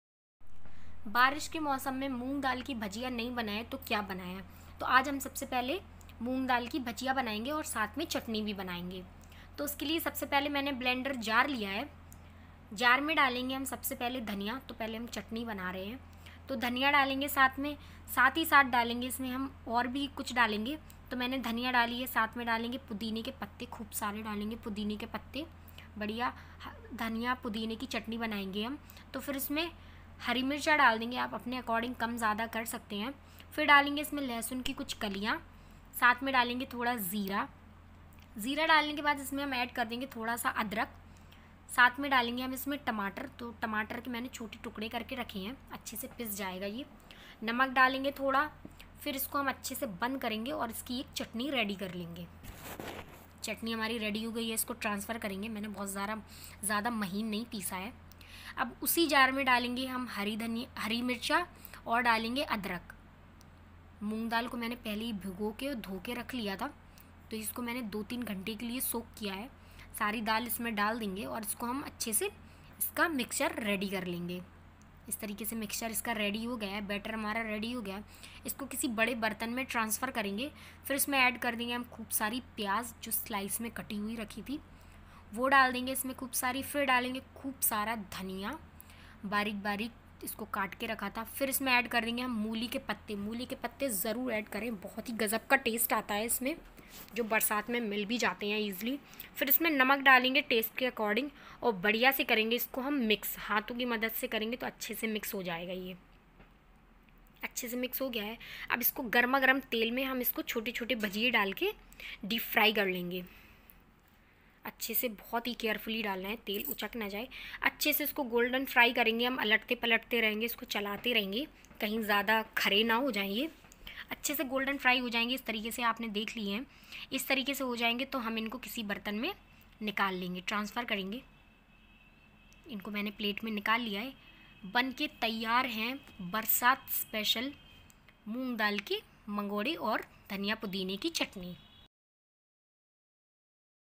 बारिश में बनाए सबके फेवरेट मूंग दाल भजिया और धनिया पुदीना की चटनी एक अलग अंदाज़ में | Monsoon special
बारिश में बनाए सबके फेवरेट मूंग दाल भजिया और धनिया पुदीना की चटनी एक अलग अंदाज़ में | Monsoon special moong dal bhajiya with dhaniya pudina chutney recipe 2024